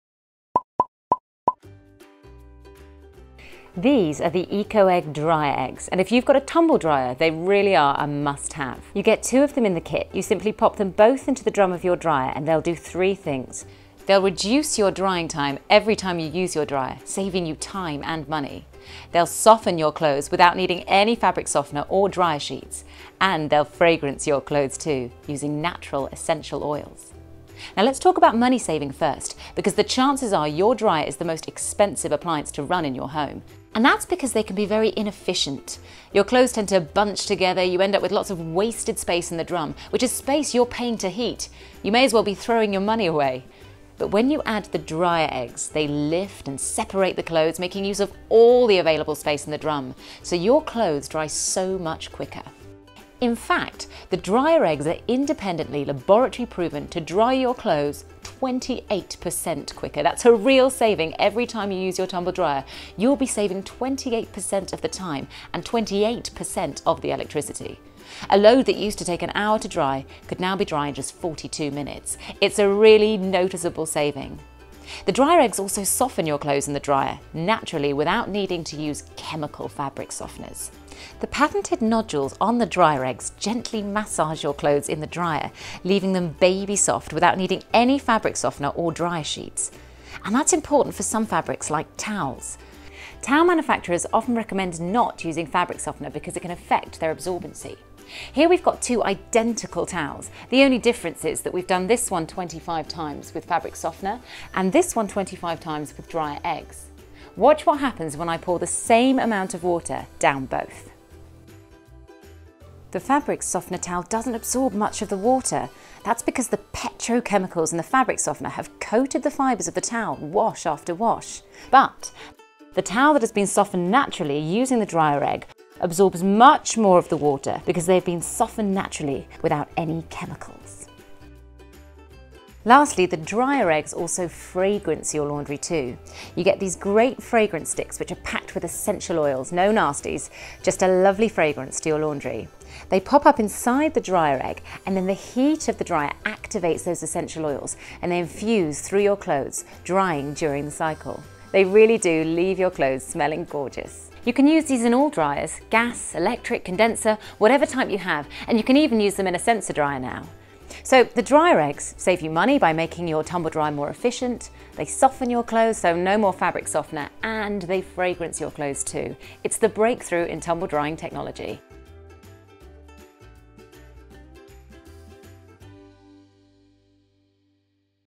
Ecoegg Spring Blossom Dryer Egg
Cut your drying time by up to 28% with the Ecoegg Dryer Egg, cleverly designed to leave clothes smelling fresh and feeling soft without the use of fabric softeners.